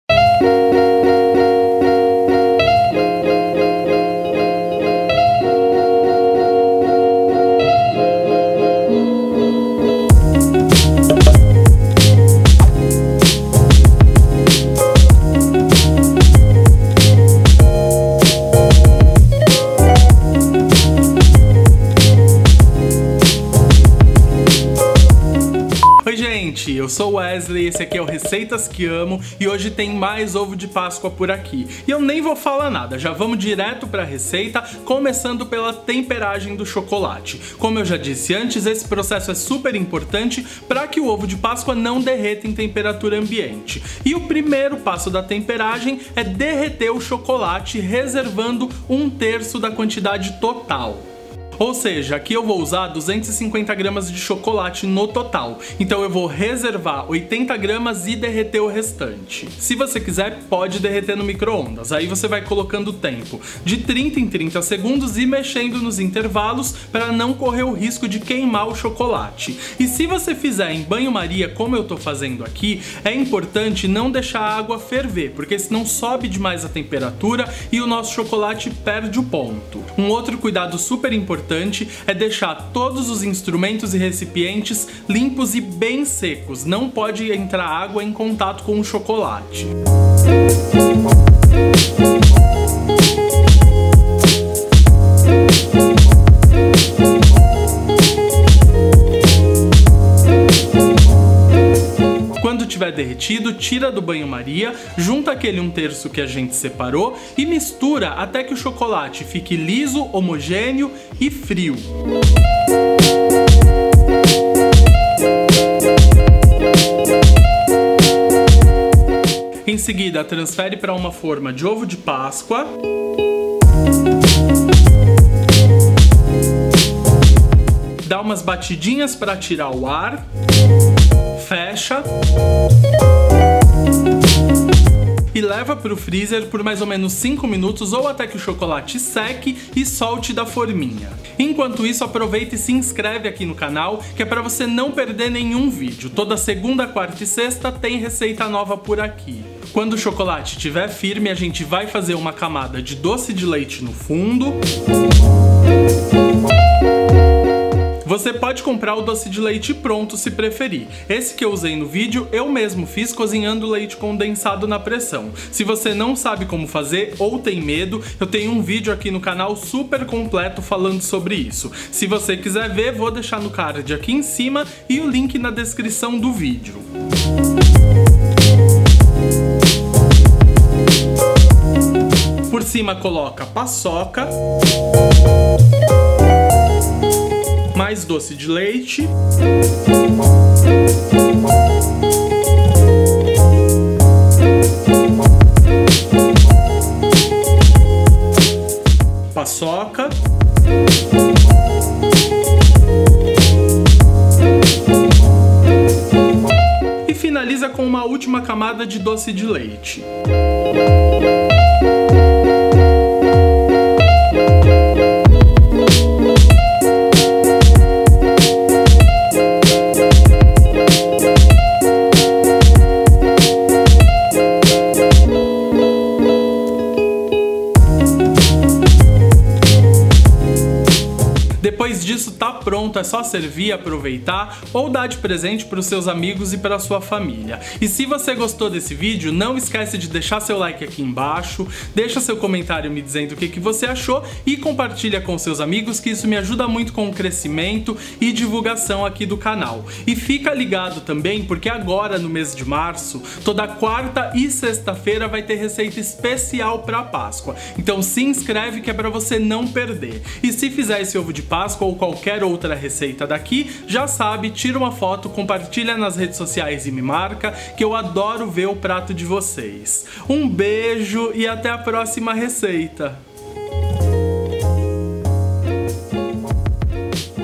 OVO DE PÁSCOA PAÇOCA - Ovo de Páscoa de colher com doce de leite - PÁSCOA 2018 - RECEITAS QUE AMO
Neste vídeo eu ensino a fazer Ovo de Páscoa de colher com doce de leite e paçoca, pode chamar de ovos de Páscoa Paçoca se preferir. Dá o play e vem ver!